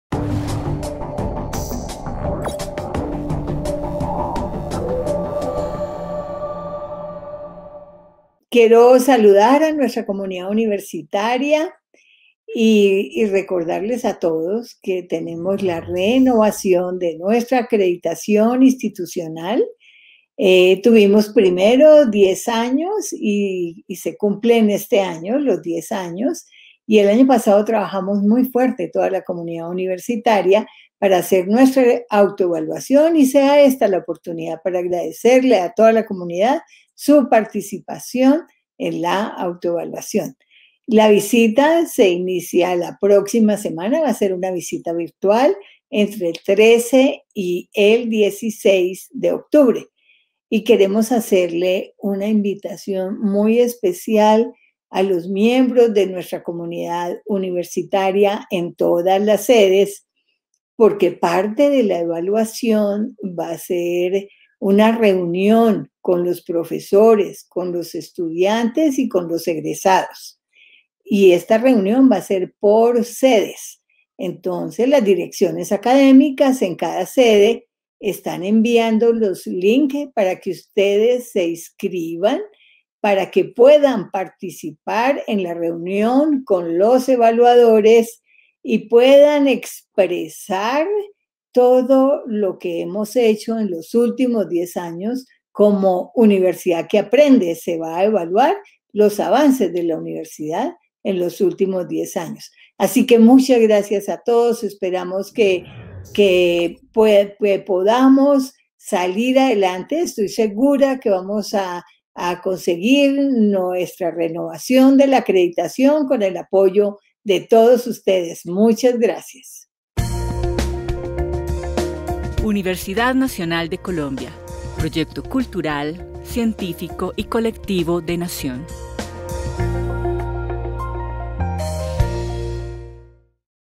SomosAcreditación | Renovación de la Acreditación Institucional
Se aproxima la visita virtual de los pares académicos para la Renovación de la Acreditación Institucional de la UNAL ¡Conéctate y avancemos unidos hacia nuestra meta! ✅